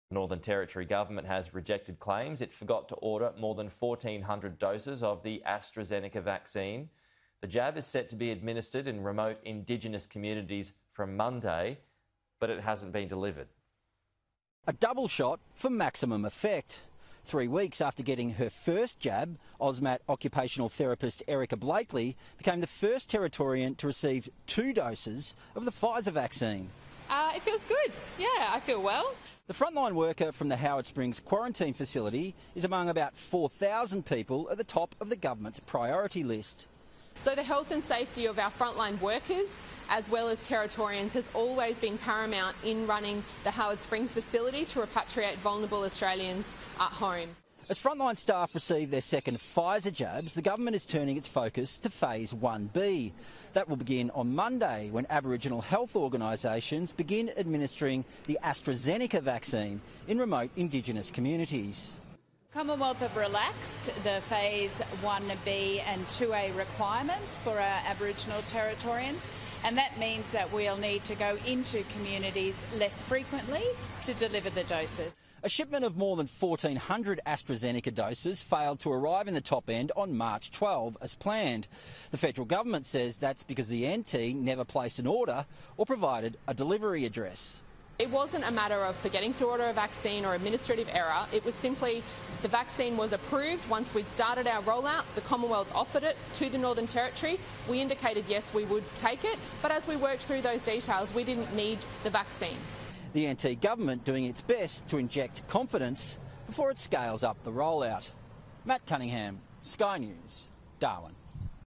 NT government denies it forgot to order vaccine shipment
The Northern Territory government has denied claims it forgot to order more doses of the COVID-19 vaccine as it prepares to administer the jab to remote Indigenous communities from next week.

As many NT frontline staff are receiving their second dose of the vaccine, the government is turning its focus to phase 1b of the rollout – which will see Indigenous health organisations begin to inoculate Indigenous Australians with the AstraZeneca jab.

A shipment of more than 1,400 doses failed to arrive in the Top End on March 12 as planned, with the federal government claiming the NT government never placed an order or submitted a delivery address.

NT Health Minister Natasha Fyles denied the assertion, however, saying “it wasn’t a matter of forgetting to order a vaccine or an administrative error”.

“It was simply the vaccine was approved once we’d started our rollout, the Commonwealth offered it to the Northern Territory, we indicated yes we would take it, but as we worked through those details we didn’t need the vaccine,” she said.